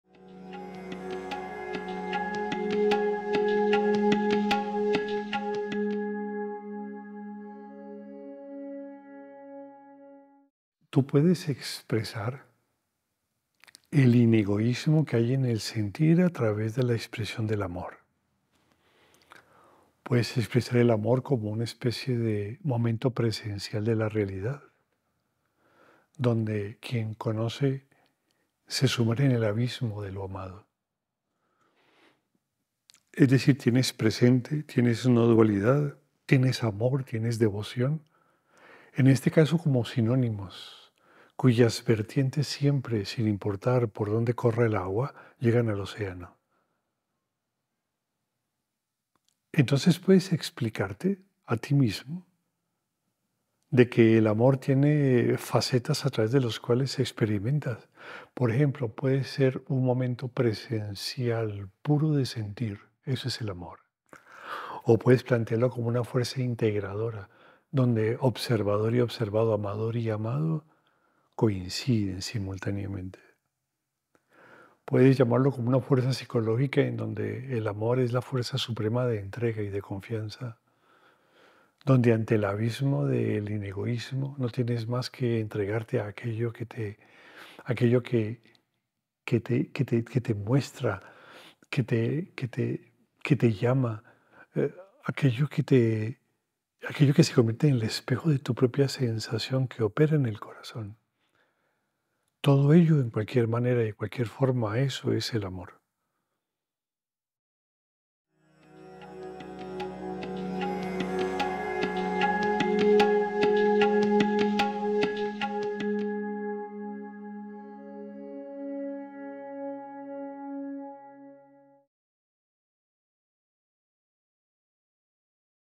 El amor
En este video, Sesha diserta acerca del inegoísmo que hay en el sentir y que se puede expresar a través del amor como fuerza integradora.

Producido por AFVAS y realizado por SerLibreMente